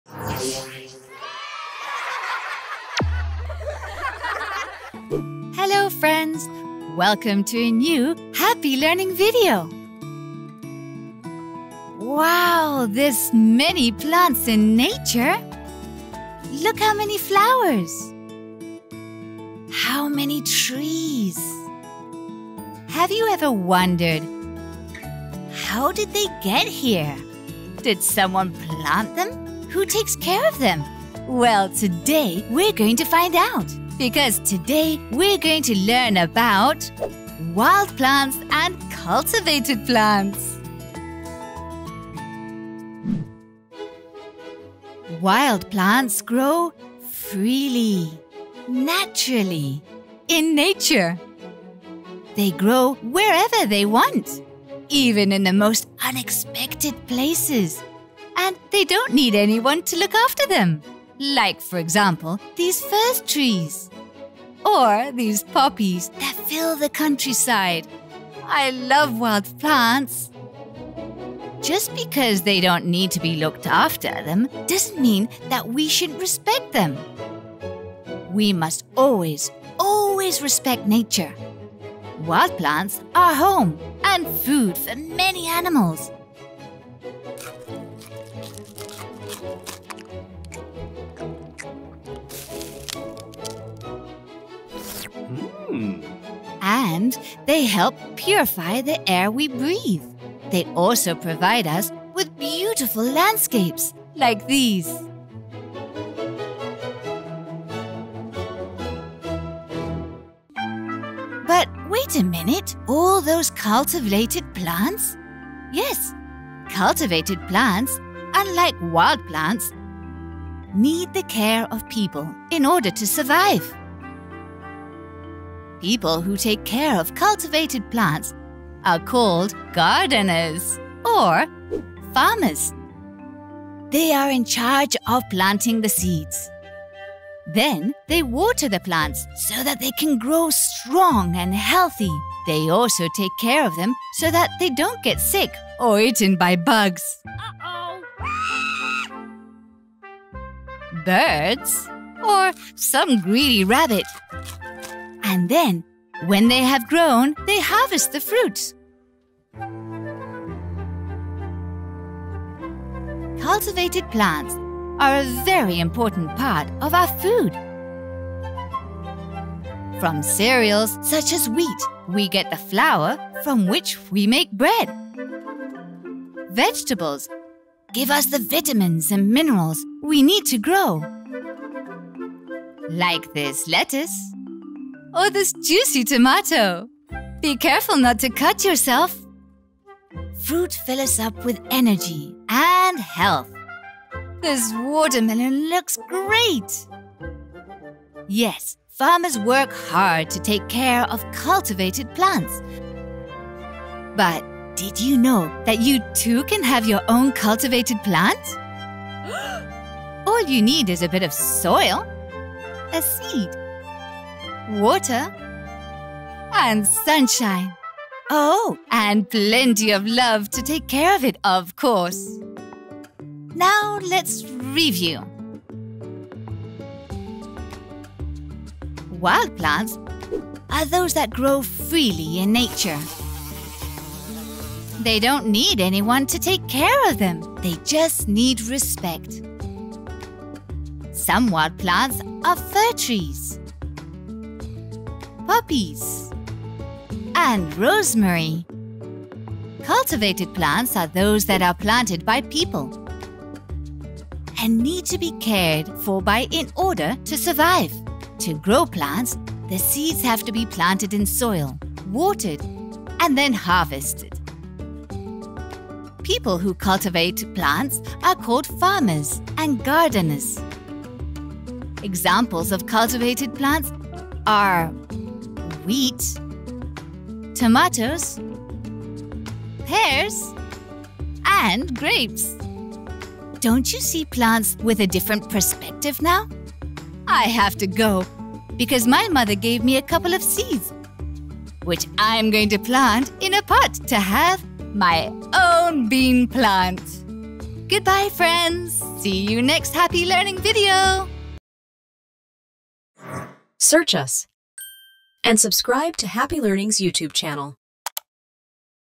🌱 Curiosities About Wild and Cultivated Plants | Educational Videos for Kids
Did you know that some plants grow on their own in nature while others need special care? Discover the differences between wild and cultivated plants in this fun video, with surprising facts about how we take care of them and why they are so important for our life! 🌸🌿